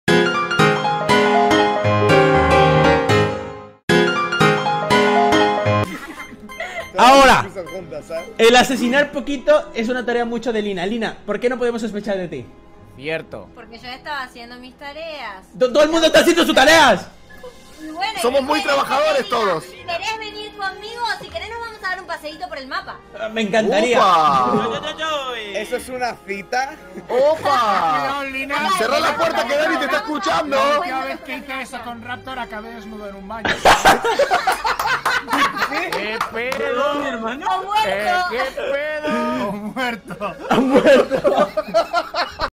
Eso es una Cita?!! 🙂 Sparta se pone celoso 🤣 ❤️💙💜 - Momento Spartor meme
Sparta se pone celoso de Lyna 🤣😅, momento Spartor en directo de Raptor